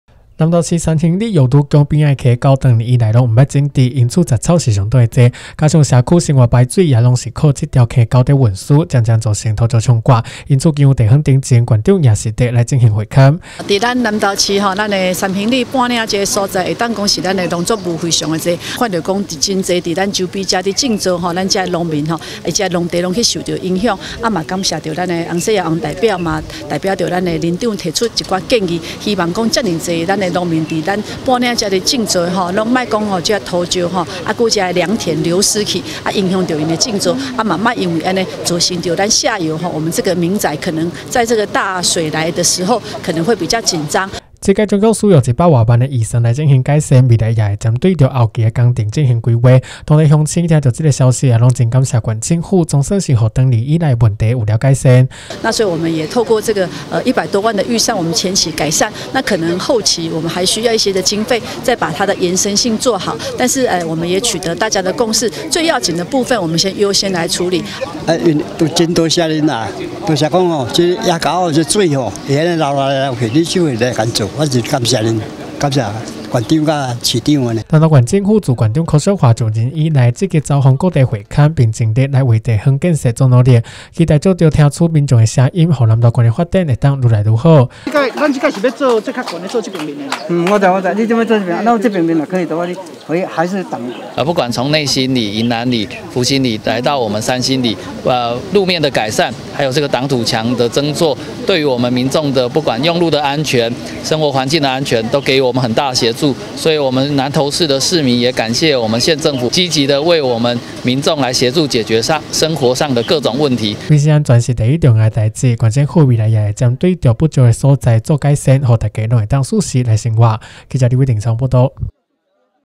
投市嶺興路坑溝土石沖刷嚴重 縣長指示改善 南投新聞
位於八卦山上三興里嶺興路玉女宮旁坑溝，多年未整治，雜草叢生，下大雨時路面水漫流，加上社區生活排水也都匯集流入，整段溝坑土石沖刷日益嚴重，因此經由地方陳情，縣長許淑華也實地進行會勘，並指示盡速發包改善。